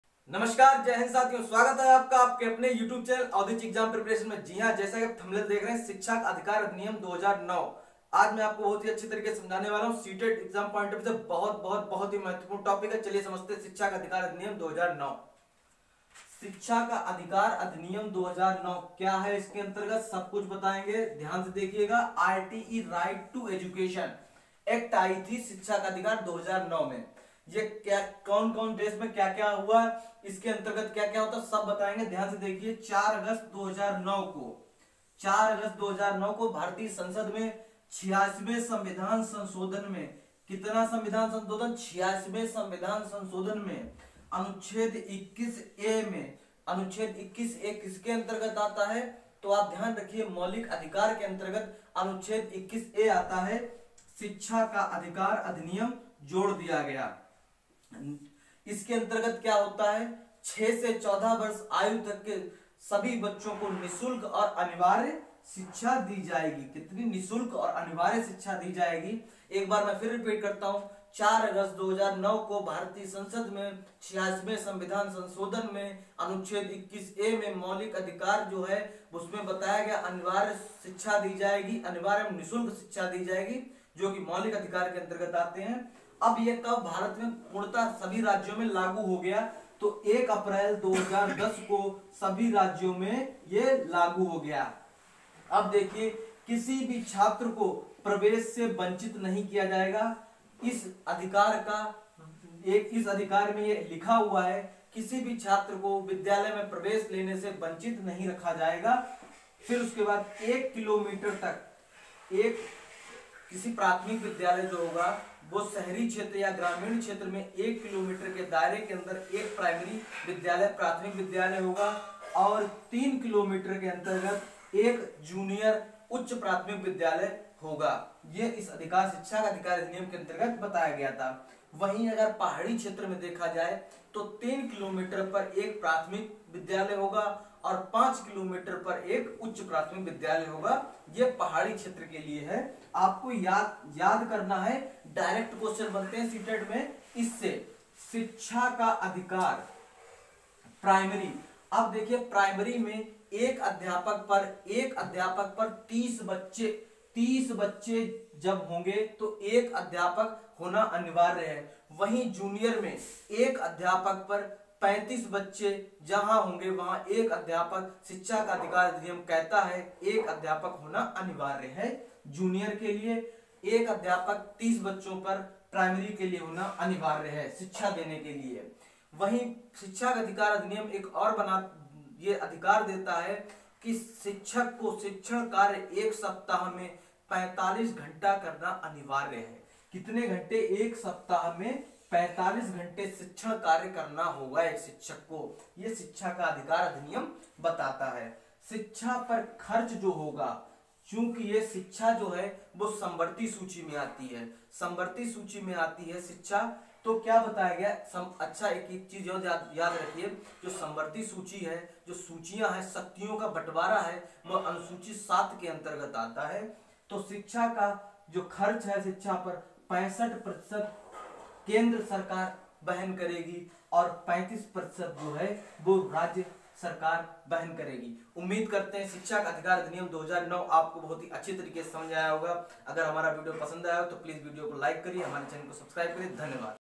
RTE-2009/शिक्षा का अधिकार अधिनियम-2009/SHIKSHA KA ADHIKAR ADHINIYAM -2009
Title:RTE-2009/शिक्षा का अधिकार अधिनियम-2009/SHIKSHA KA ADHIKAR ADHINIYAM -2009

Audichya exam preparation#

About this video:-  साथियों इस वीडियो के अंतर्गत शिक्षा का अधिकार अधिनियम 2009 को विस्तार से समझाया गया है।
WhatsApp group for study material and all notification 👇👇🚩🇮🇳